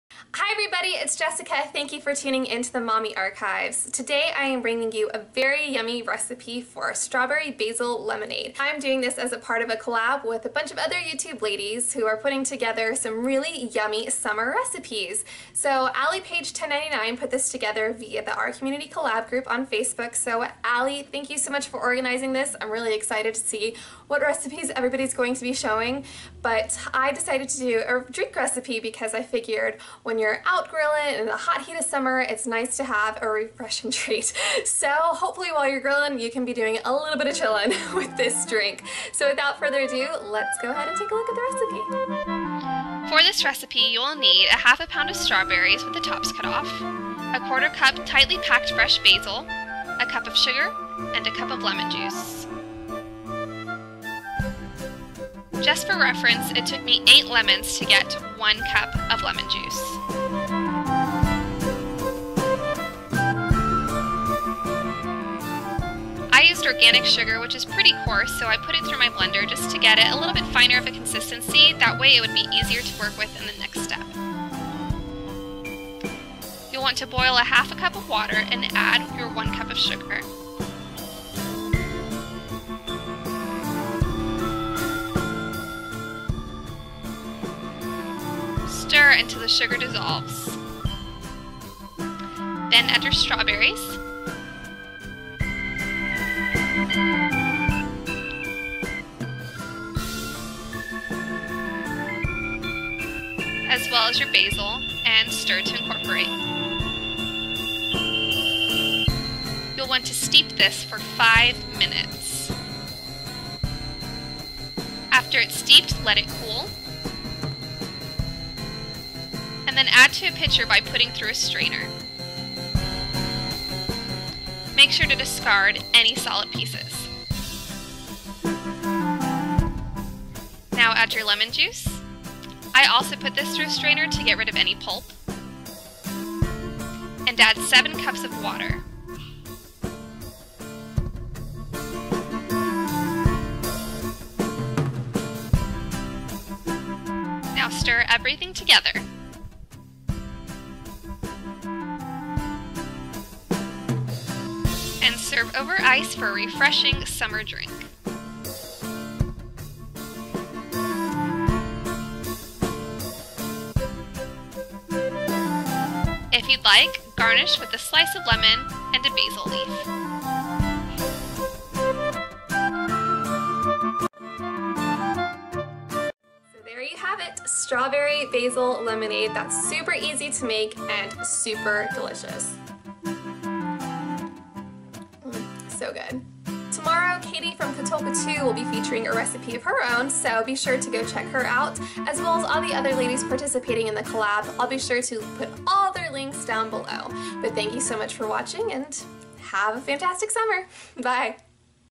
Strawberry Basil Lemonade Recipe
I'm excited to bring you a delicious recipe for strawberry basil lemonade as part of the Grillin' & Chillin' Collab.  It's the perfect drink for a scorching summer day when you're looking for something refreshing.  Strawberry basil lemonade is a yummy twist on an old classic, and it's great for entertaining when you have friends or family over for some summer grillin' fun.

Special thanks to AlliePaige1099 for organizing this via the Our Community Collab Facebook page.  Tomorrow, Kaity from Katulka2 will be contributing.